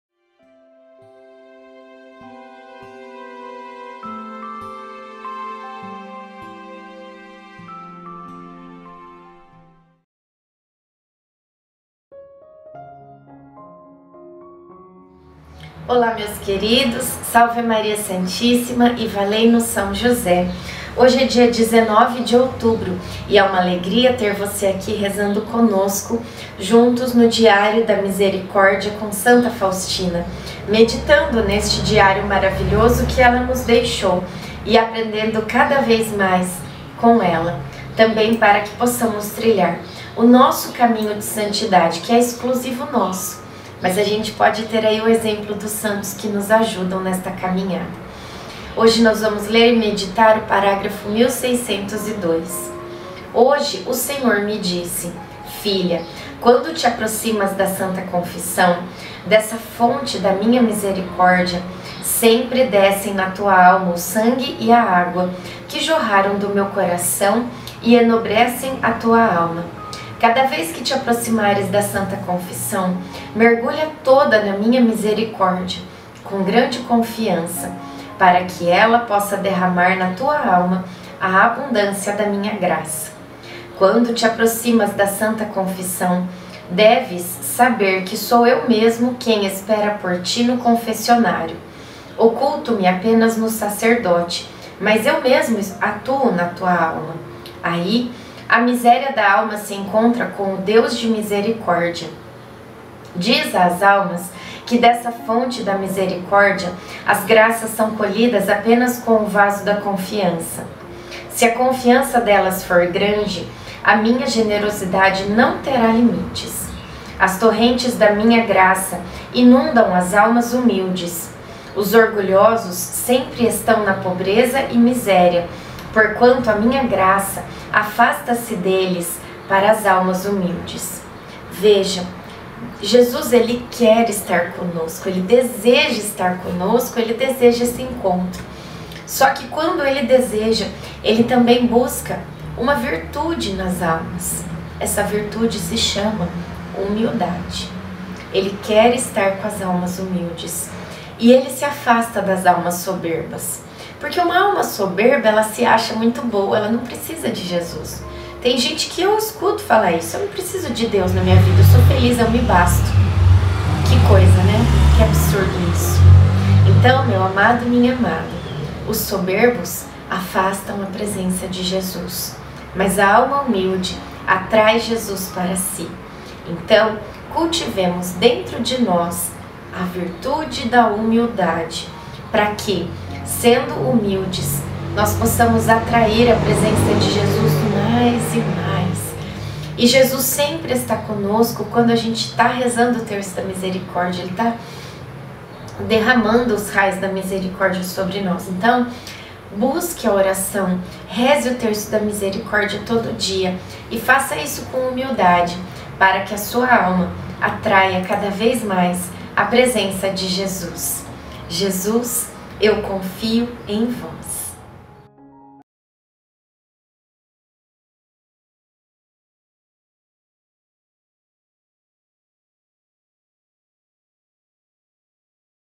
DIÁRIO DA MISERICÓRDIA COM SANTA FAUSTINA 19/10/24
1602Hoje, o Senhor me disse: Filha, quando te aproximas da santa
confissão, dessa fonte da Minha misericórdia, sempre descem
na tua alma o Sangue e a Água que jorraram do Meu Coração
e enobrecem a tua alma. Cada vez que te aproximares da santa
confissão, mergulha toda na Minha misericórdia com grande
confiança, para que Ela possa derramar na tua alma a
abundância da Minha graça. Quando te aproximas da santa
confissão, deves saber que sou Eu mesmo quem espera por ti no
confessionário; oculto-Me apenas no sacerdote, mas Eu mesmo
atuo na alma. Aí, a miséria da alma se encontra com o Deus de
Misericórdia. Diz às almas que dessa fonte da misericórdia (7)
as graças são colhidas apenas com o vaso da confiança. Se a
confiança delas for grande, a Minha generosidade não terá
limites. As torrentes da Minha graça inundam as almas
humildes. Os orgulhosos sempre estão na pobreza e miséria,
porquanto a Minha graça afasta-se deles para as almas
humildes.

Deus abençoe!